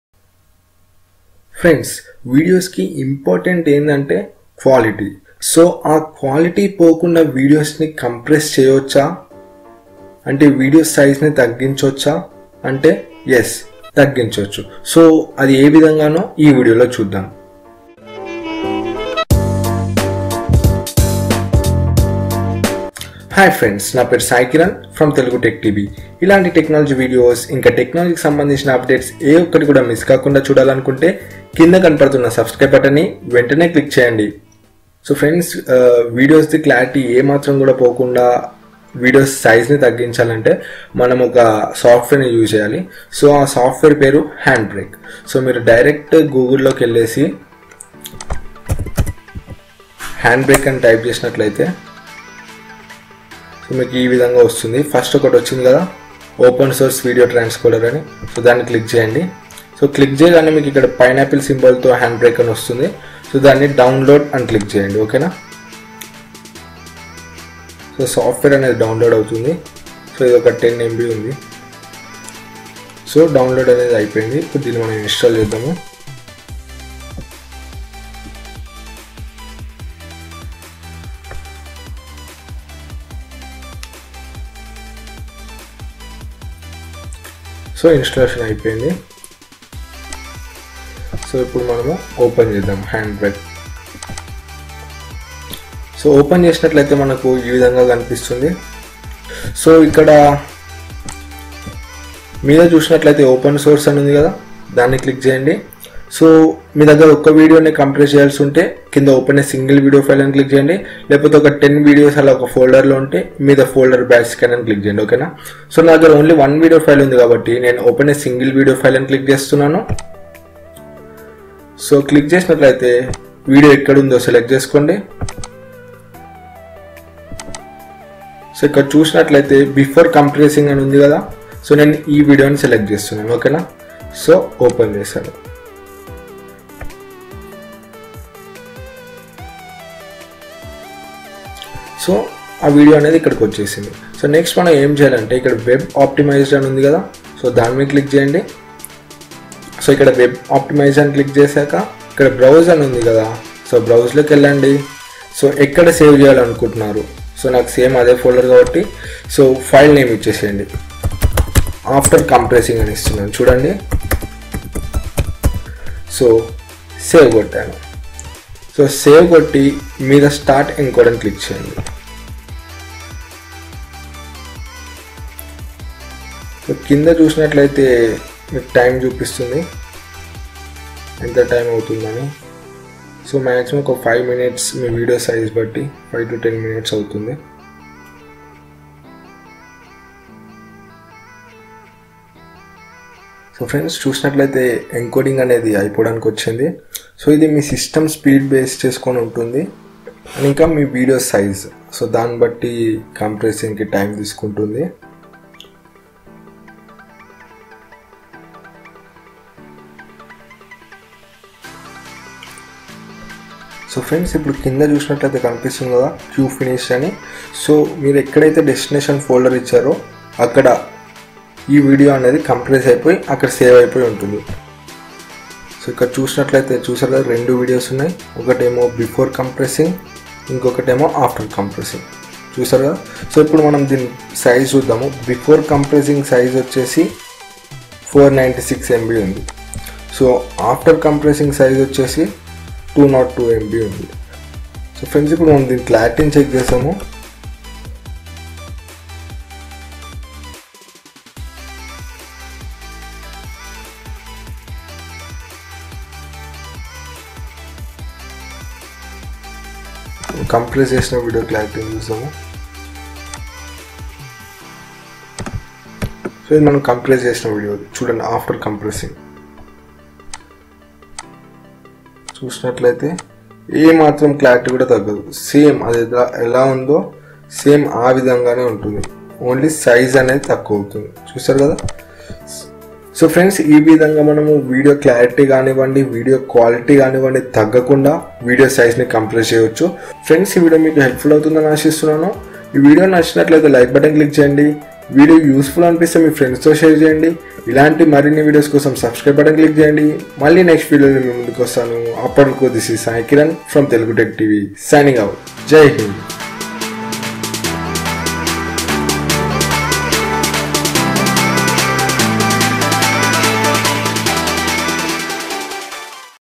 How to Compress Video Size Without Losing Clarity | Decrease Video Size | In Telugu
How to Compress any video without loosing quality Explained in telugu
Hello Telugu Tech geeks in this video iam going to show how to reduce video size to half of its size without losing clarity